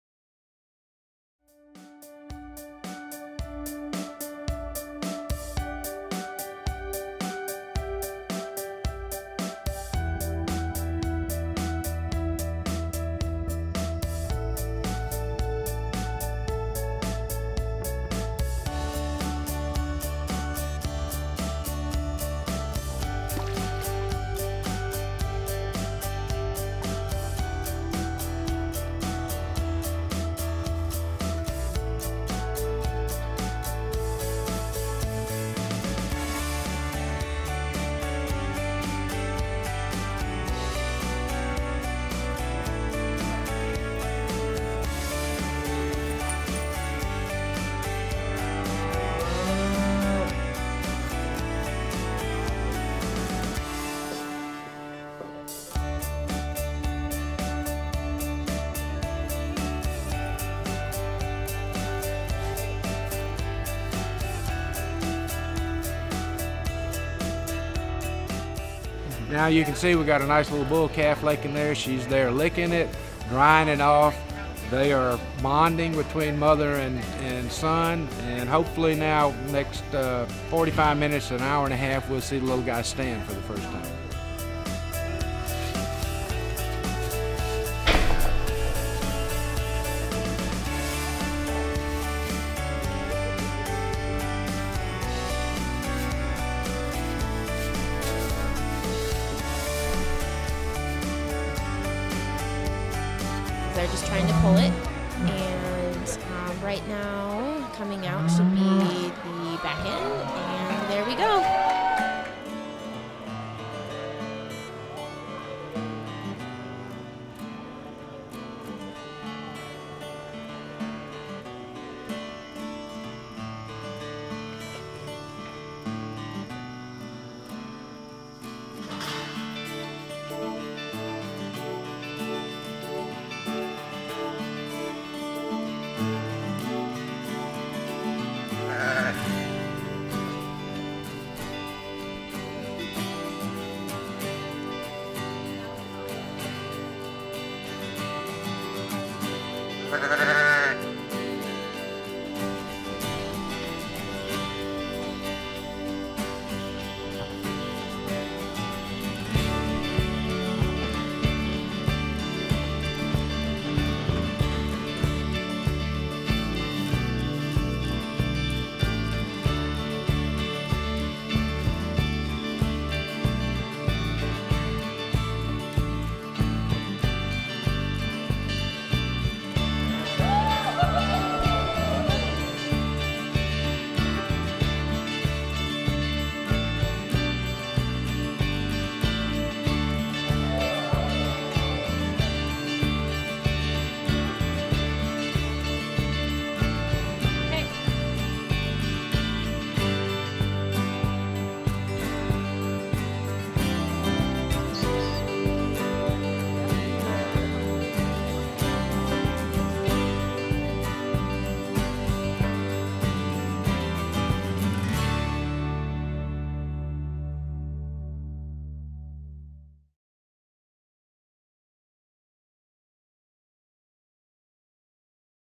Calves, Lambs and Piglets Born Before Your Eyes at LaborLive
Animal Planet's live cam site, APL!VE (apl.tv), captured animal births LIVE at the Nebraska State Fair. See the amazing highlights.

Check out the Animalist App!